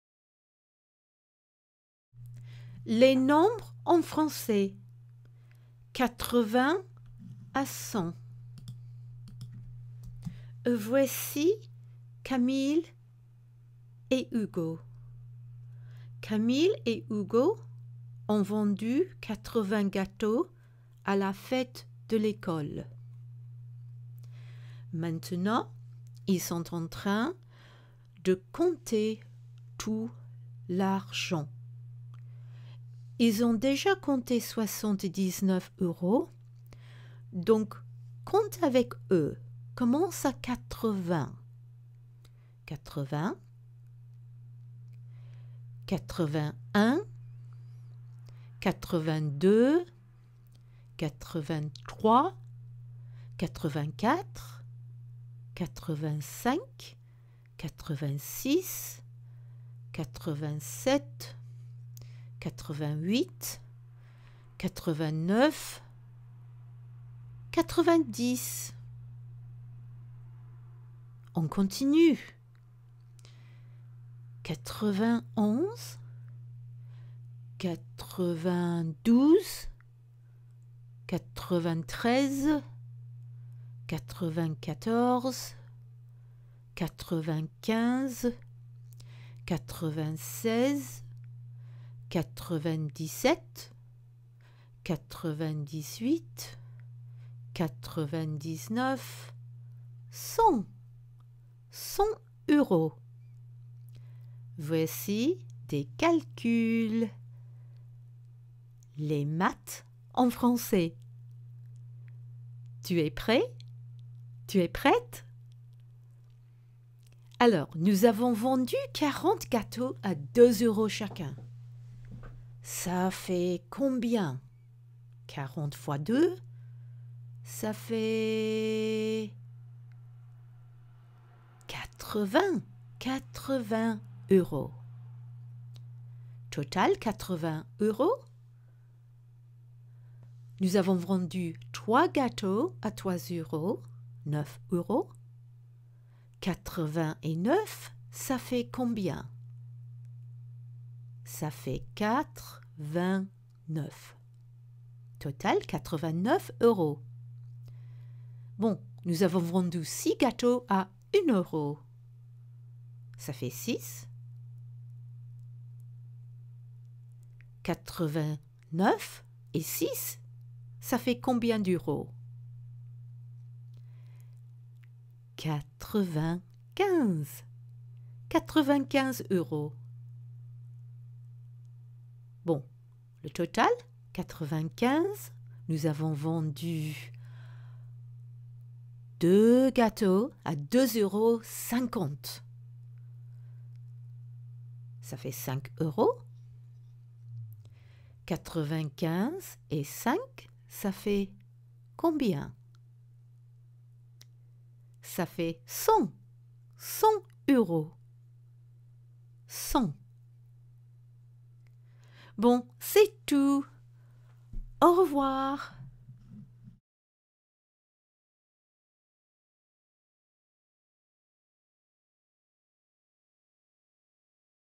FRENCH NUMBERS 80 to 100 - Nombres en français 80 à 100 -Effortless learning
Learn numbers in French from 80 to 100 and  sharpen your linguistic and mathematical skills with  interactive simple calculations.

Learn other useful French vocabulary as you count from 80 to 100.